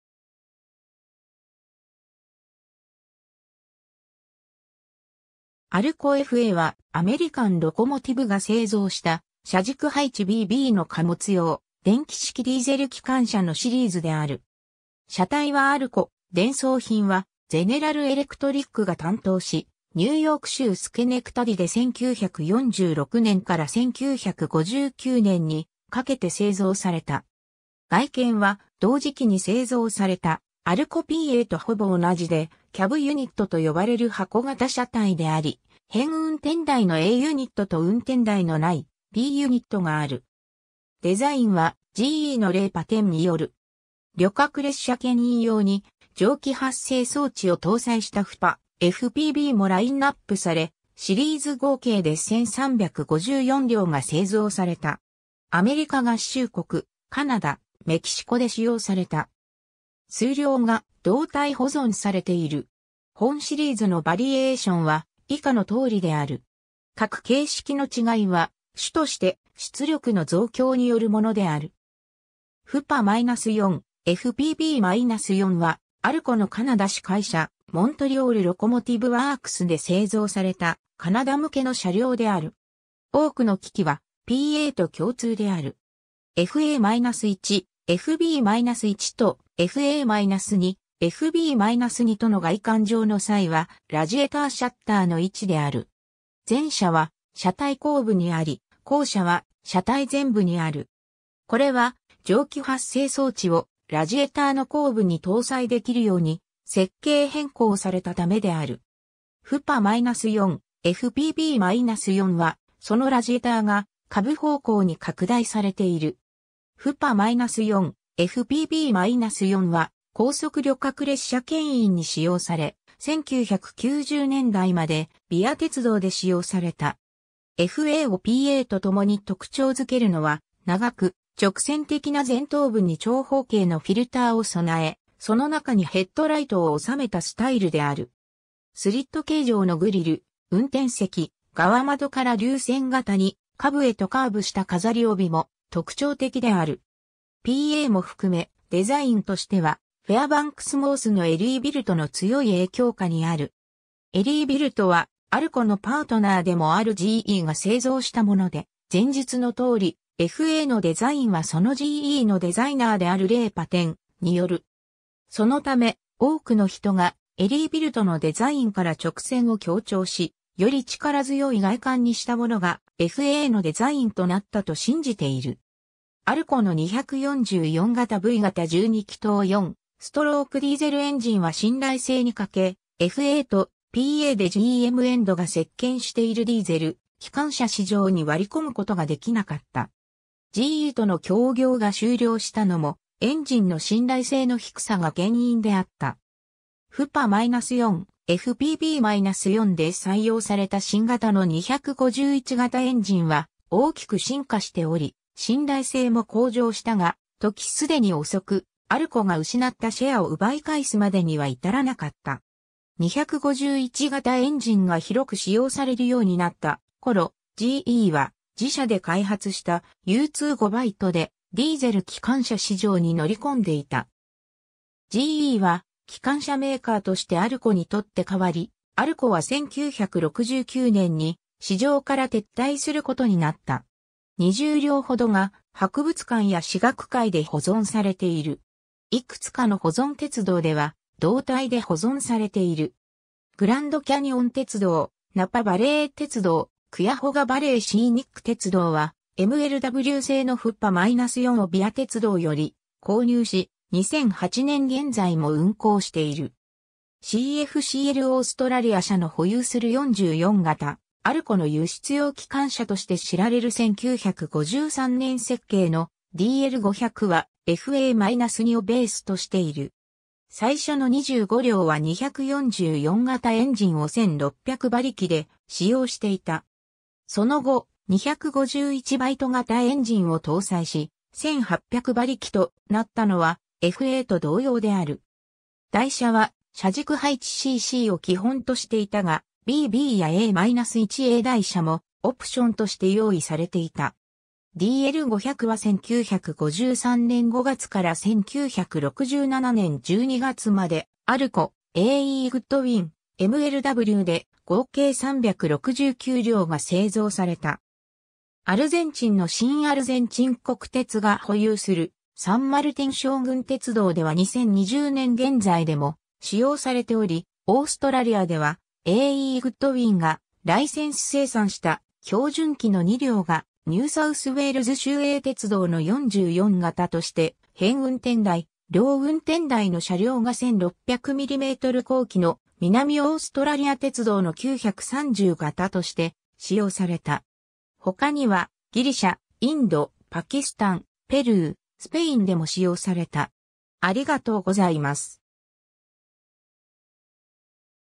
アルコFA
アメリカン・ロコモティブ製のディーゼル機関車
アルコFAは、アメリカン・ロコモティブ（アルコ）が製造した車軸配置B-Bの貨物用電気式ディーゼル機関車のシリーズである。
車体はアルコ、電装品はゼネラル・エレクトリック(GE)が担当し、ニューヨーク州スケネクタディで1946年から1959年にかけて製造された。
 外見は同時期に製造されたアルコPAとほぼ同じで、キャブ・ユニットと呼ばれる箱形車体であり、片運転台のAユニット(FA)と運転台のないBユニット(FB)がある。
デザインはGEのレイ・パテン(Ray Patten)による。
 旅客列車牽引用に蒸気発生装置を搭載したFPA/FPBもラインナップされ、シリーズ合計で1,354両が製造された。
アメリカ合衆国、カナダ、メキシコで使用された。
数両が動態保存されている。
 本シリーズのバリエーションは以下の通りである。
 各形式の違いは、主として出力の増強によるものである。
FPA-4/FPB-4は、アルコのカナダ子会社、モントリオール・ロコモティブ・ワークス(MLW)で製造された、カナダ向けの車両である。
 多くの機器はPAと共通である。
FA-1/FB-1とFA-2/FB-2との外観上の差異は、ラジエターシャッターの位置である。
前者は車体後部にあり、後者は車体前部にある。
これは、蒸気発生装置をラジエターの後部に搭載できるように設計変更されたためである。
FPA-4/FPB-4は、そのラジエターが下部方向に拡大されている。
FPA-4/FPB-4は高速旅客列車牽引に使用され、1990年代までVIA鉄道で使用された。
 FAをPAとともに特徴づけるのは、長く直線的な前頭部に長方形のフィルターを備え、その中にヘッドライトを収めたスタイルである。
スリット形状のグリル、運転席側窓から流線型に下部へとカーブした飾り帯も特徴的である。
 PAも含め、デザインとしてはフェアバンクス・モースのエリービルトの強い影響下にある。
エリービルトはアルコのパートナーでもあるGEが製造したもので、前述のとおり、FAのデザインはそのGEのデザイナーであるレイ・パテンによる。
そのため、多くの人が、エリービルトのデザインから直線を強調し、より力強い外観にしたものがFAのデザインとなったと信じている。
 アルコの244型V型12気筒4ストロークディーゼルエンジン（131,288cc、直径229mm×行程267mm）は信頼性に欠け、FAとPAでGM-EMDが席巻しているディーゼル機関車市場に割り込むことができなかった。
GEとの協業が終了したのも、エンジンの信頼性の低さが原因であった。
FPA-4/FPB-4で採用された新型の251型エンジンは大きく進化しており、信頼性も向上したが、時既に遅く、アルコが失ったシェアを奪い返すまでには至らなかった。
251型エンジンが広く使用されるようになったころ、GEは自社で開発したU25Bでディーゼル機関車市場に乗り込んでいた。
GEは機関車メーカーとしてアルコに取って代わり、アルコは1969年に市場から撤退することになった。
 20両ほどが、博物館や史学会で保存されている。
いくつかの保存鉄道では動態で保存されている。
グランド・キャニオン鉄道、ナパ・バレー鉄道、クヤホガ・バレー・シーニック鉄道はMLW製のFPA-4をVIA鉄道より購入し、2008年現在も運行している。
 CFCLオーストラリア社の保有する44形 アルコの輸出用機関車として知られる1953年設計のDL500は、FA-2をベースとしている。
最初の25両は244型エンジンを1,600馬力(1,200kW)で使用していた。
その後、251B型エンジンを搭載し、1,800馬力(1,300kW)となったのはFAと同様である。
台車は車軸配置C-Cを基本としていたが、B-BやA-1-A台車もオプションとして用意されていた。
 DL500は1953年5月から1967年12月まで、アルコ、A.E.グッドウィン、MLWで合計369両が製造された。
 アルゼンチンの新アルゼンチン国鉄が保有するサン・マルティン将軍鉄道では2020年現在でも使用されており、オーストラリアではA.E.グッドウィンがライセンス生産した標準軌の2両がニューサウスウェールズ州営鉄道(New S...